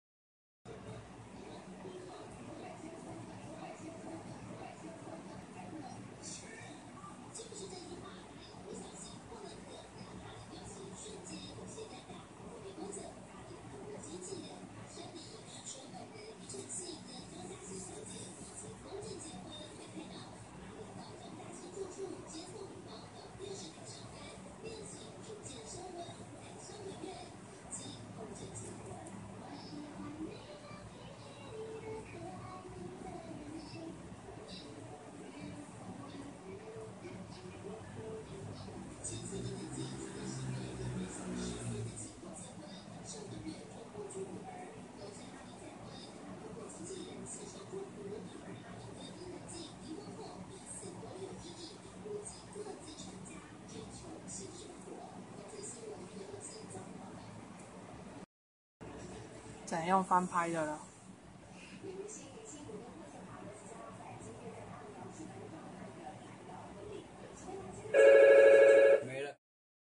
救護員急駛30公里 孕婦救護車上生產
宜蘭縣消防局南澳分隊到蘇澳鎮朝陽里搭載孕婦，救護車開在蘇花公路時，女子在救護車上生產，救護人員協助接生，發現新生兒臍帶繞頸，小心翼翼解開臍帶，行駛30公里路程後送達羅東聖母醫院，圓滿達成急產救護任務。